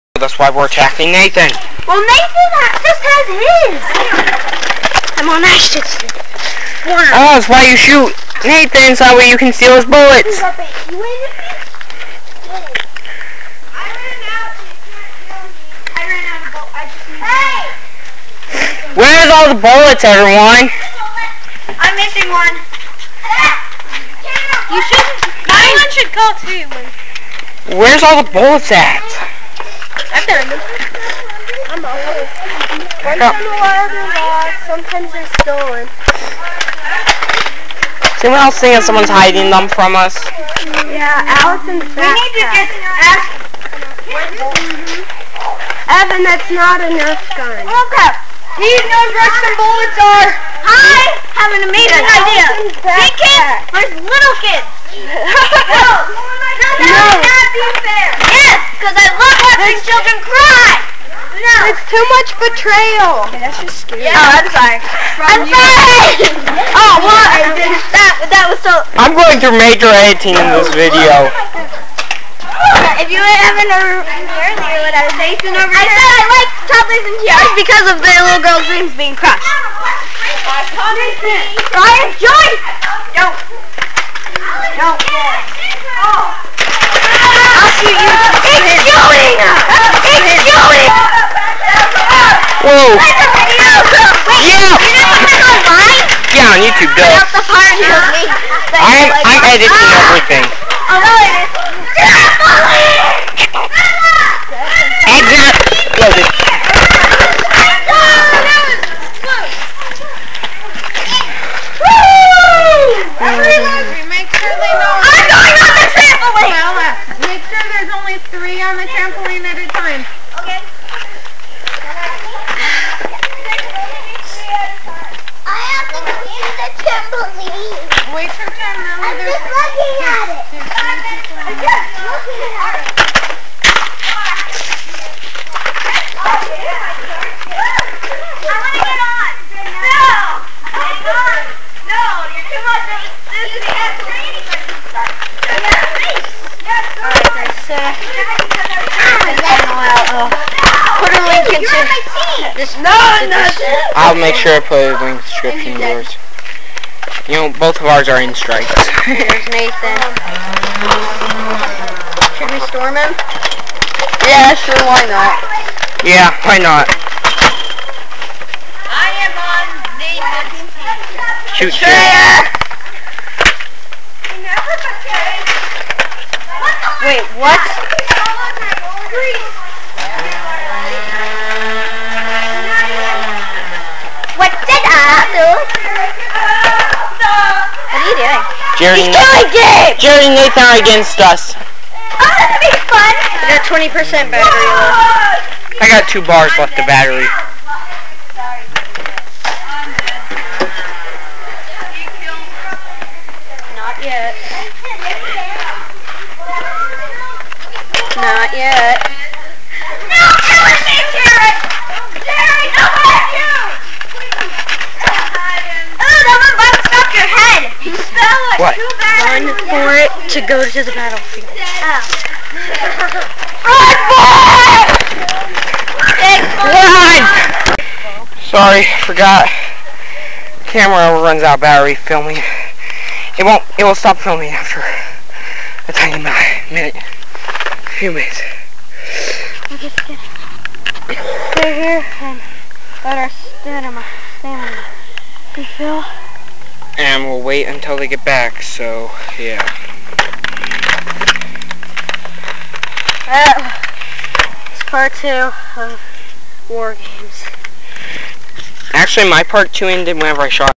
War Games Season Finaily Part 2
The gain and I continue to fight Mr. Ocelot to get the bullets and DigDaDiamond and i go on a quest to hide.